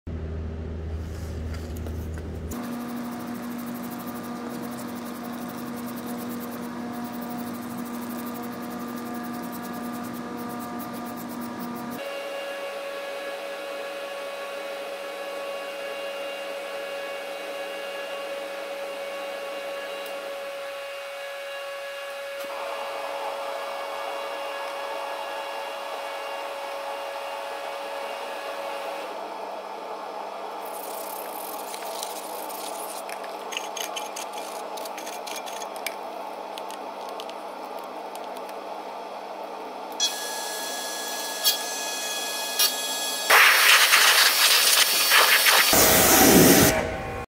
Nasty Wheel TRANSFORMATION with Turtle Wax HyperFoam cleaner!
This new wheel cleaner from Turtle Wax is awesome. Great at removing tire grime and also at attacking brake dust on rims.